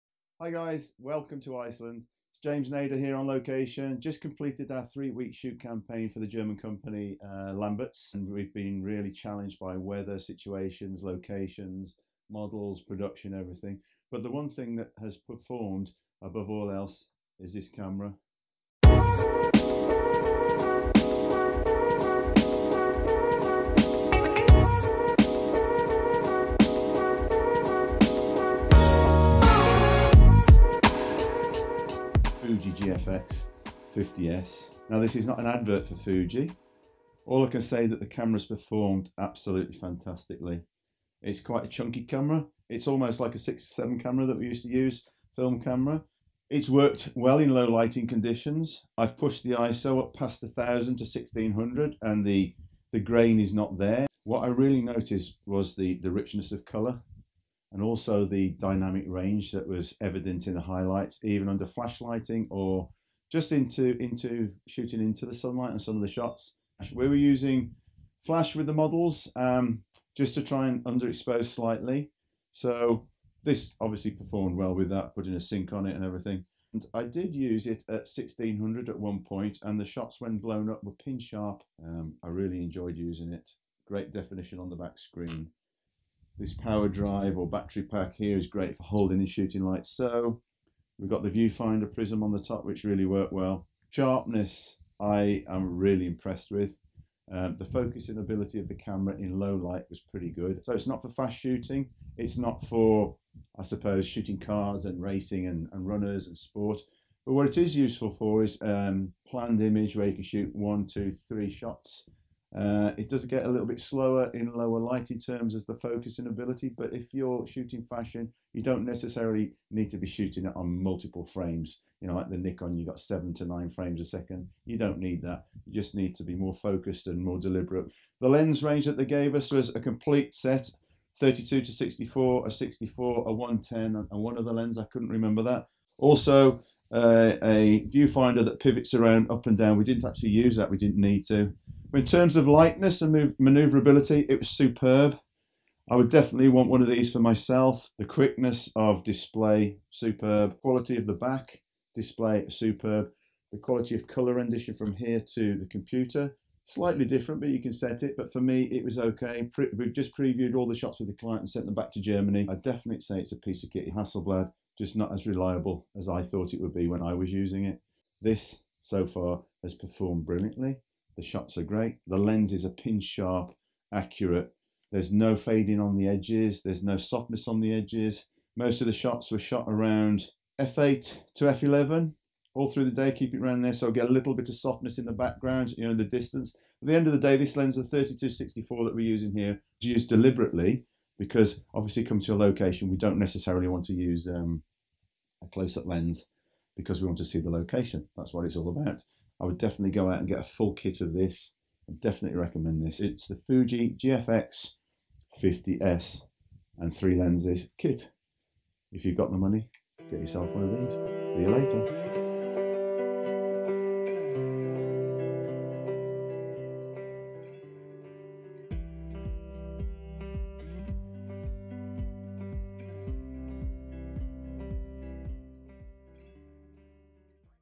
FUJI CAMERA GFX50S Hands James Nader Feedback ON LOCATION ICELAND
LOCATION ICELAND FEEDBACK ON THE FUJI GFX50S
Fuji Cameras - My first Fuji Camera was the S2-Pro a few years ago now and how the cameras are proving to be a superb alternative to both Hasselblad and Phase One.
GFX50S - Hands-on Field Test results.
A Camera for All Seasons - The Fuji GFX50S one of the best cameras and I think better than the Hasselblad.
This video summarises in the field experience with the Fuji GFX50S by @fujifilm_uk after a 3-week fashion and calendar assignment in the land of Fire and Ice that is Iceland.